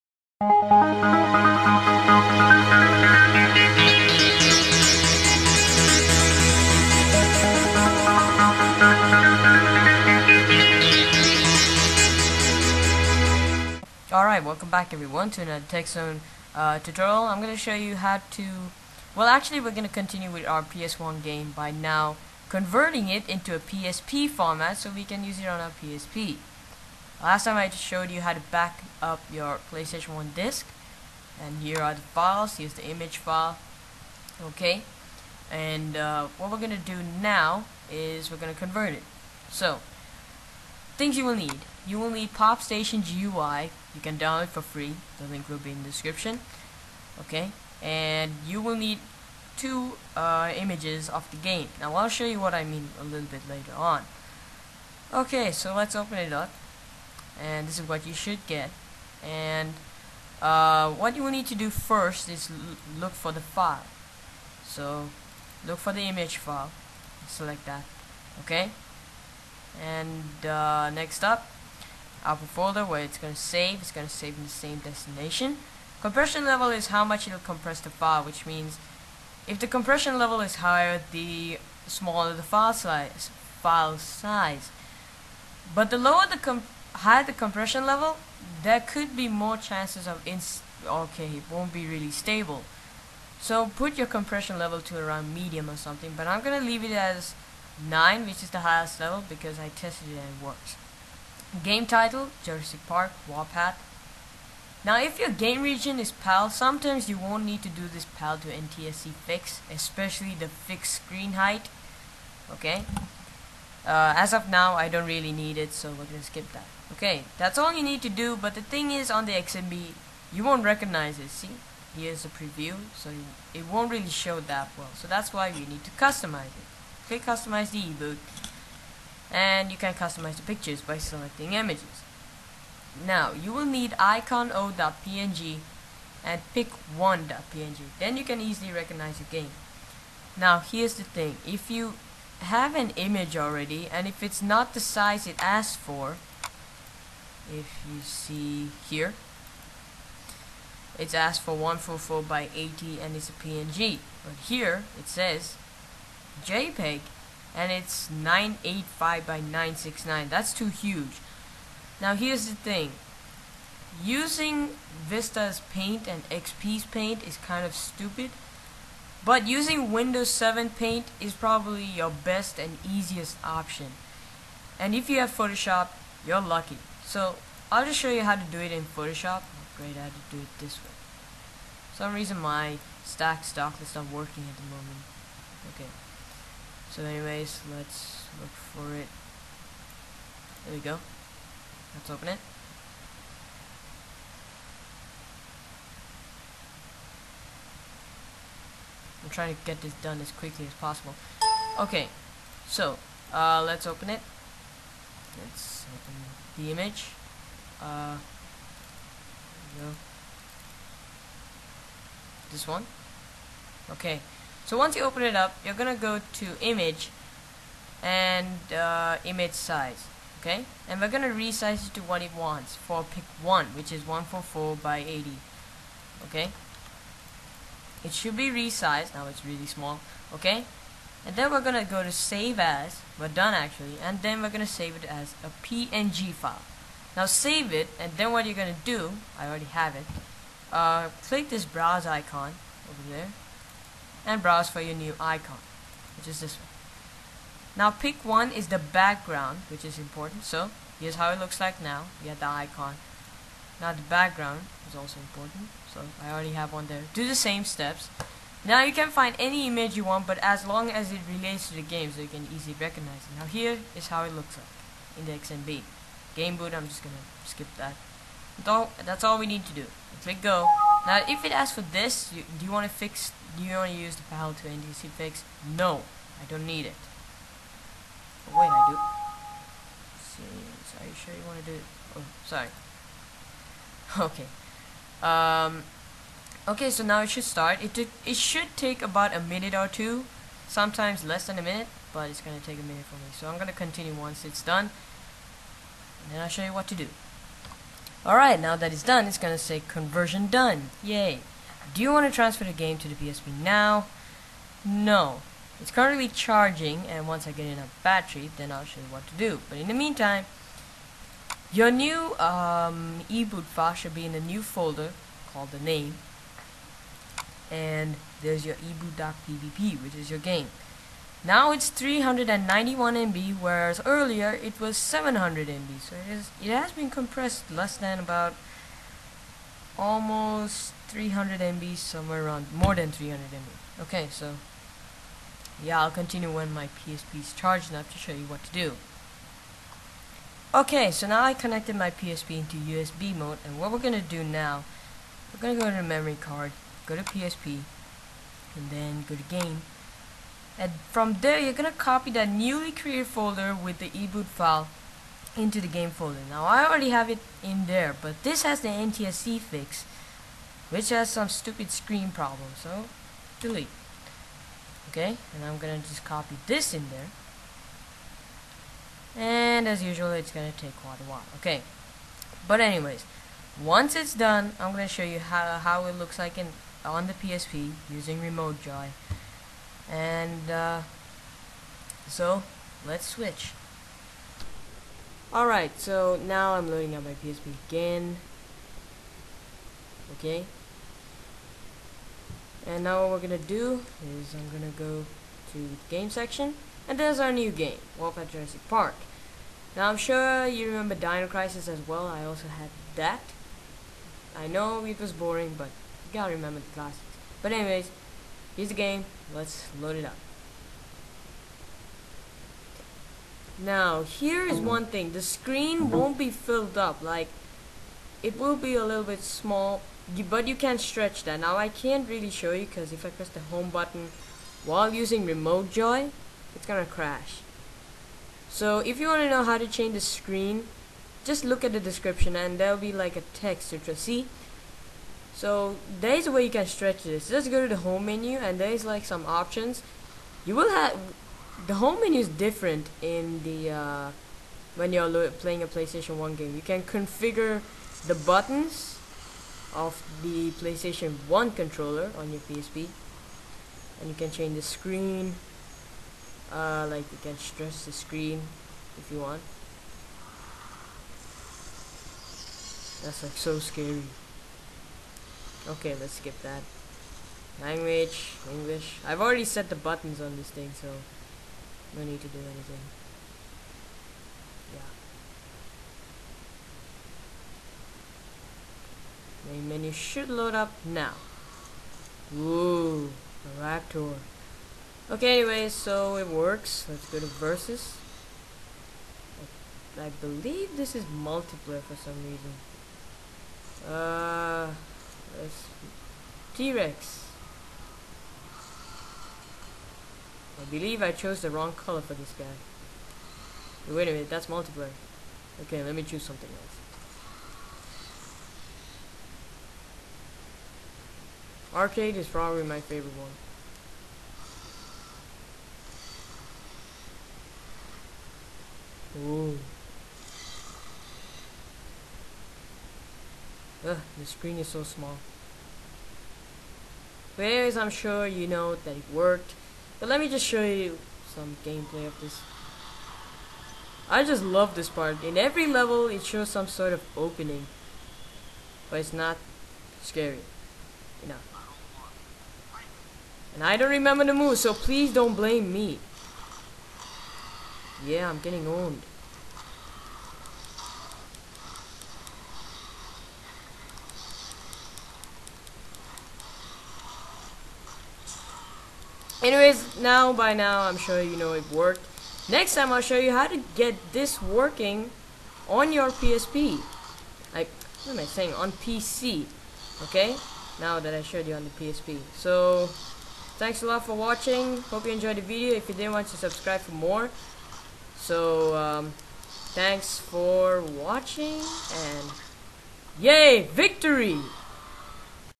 Play PS1 Backups On PSP
In my last tutorial, i  showed you how to backup your PS1 game to your computer. Well now, let's convert to a suitable format for use with our PSP. Alright, you will need the program, POPStationGUI to convert the image file and then follow the instructions -

To fit the screen size, load up the game, press the 'HOME' button to bring up the settings. Go to 'Other Settings' and change the Screen Mode to 'Full Screen'